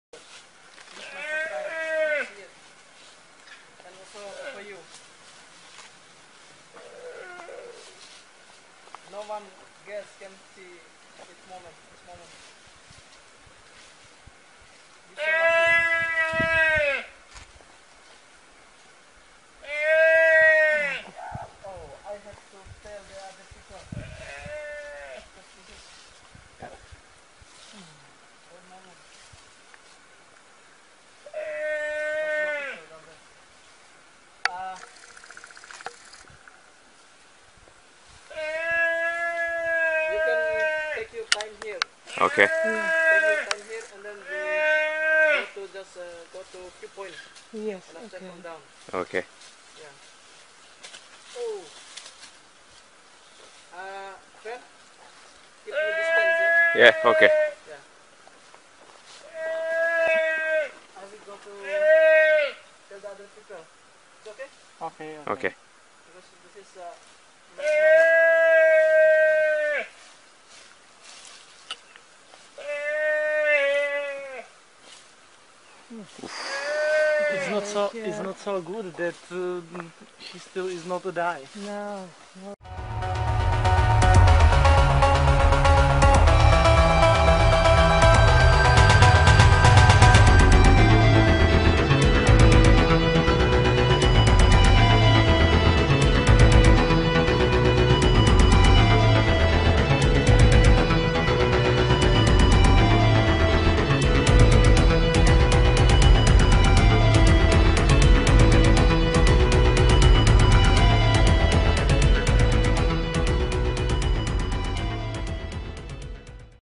#4 eaten ALIVE UNCENSORED 18+ Komodo Dragons eating a calf alive Screaming live feeding
Warning: Although this video only depicts the life in the wilderness, the scenes might be quite gruesome for sensitive users.
In this video, you can find a series of footage of Komodo Dragons which are one of the top predators.
Angry Jellyfish TV: Terrific Animal Attacks / UNCENSORED +18 - eaten ALIVE - Komodo Dragons eating a calf alive brutally- Screaming -GRAPHIC CONTENT-